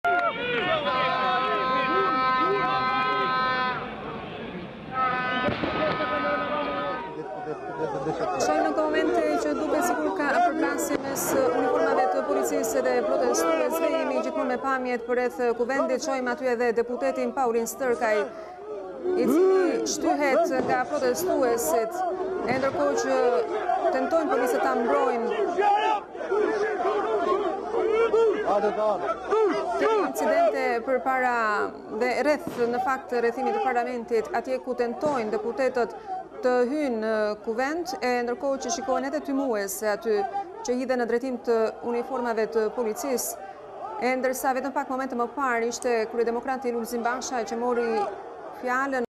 Protestuesit sulmojnë edhe deputetin Paulin Stërkaj para Parlamentit
Protestuesit e opozitës kanë sulmuar edhe deputetin Paulin Stërkaj, teksa ky i fundit futej për në Kuvend për t'ju bashkuar seancës plenare.

Protestuesit e kanë shtyrë dhe sulmuar deputetin, për të penguar futjen e tij në Kuvend. Me ndërhyrjen e forcave të policisë, deputeti ka shpëtuar nga turma.